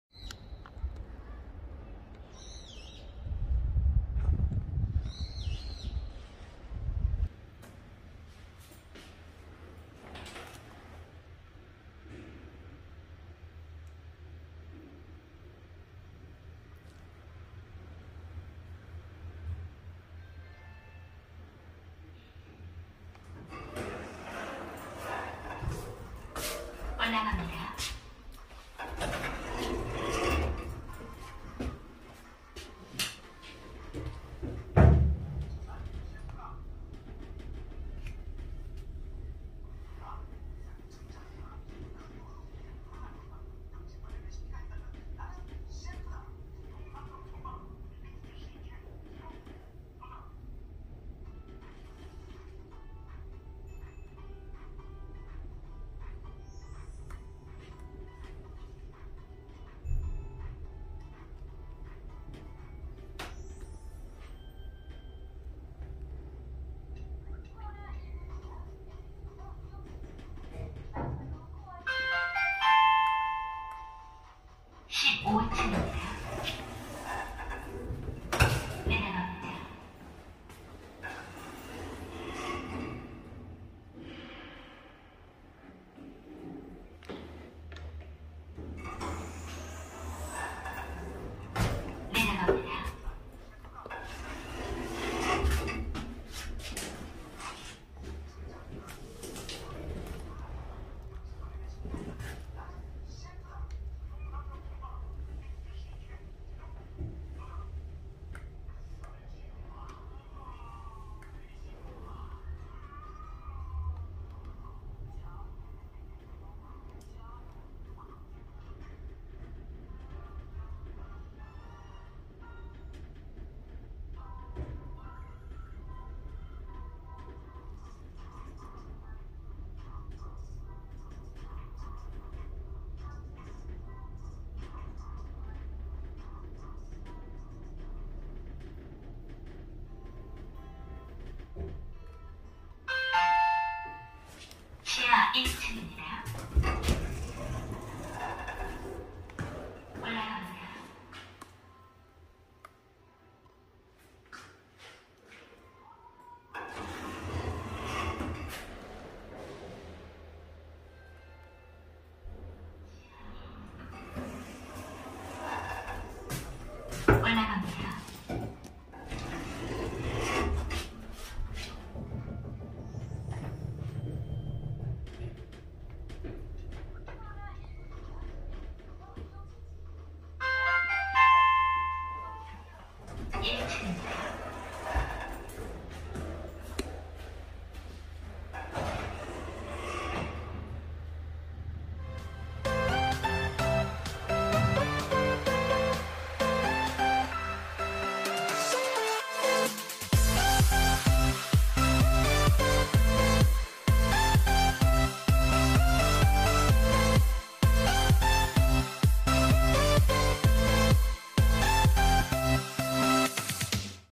[2005 DY30A] 경기도 화성시 송산동 한승미메이드1차 티센크루프 동양엘리베이터 DY30A (102동 1호기)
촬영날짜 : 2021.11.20
촬영기기 : 갤럭시 노트20 울트라
위치 : 경기도 화성시 화산중앙로 34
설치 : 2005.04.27
용량 : 15인승 1000KG
속도 : 60m/min
형식 : 권상식-VVVF
모델 : DY30A
제조사 : 티센크루프동양엘리베이터(주)
번호 : 2009-373
운행층 : B1~15F
종류 : 장애/승객화물용
보수업체 : 티케이엘리베이터코리아(주)경원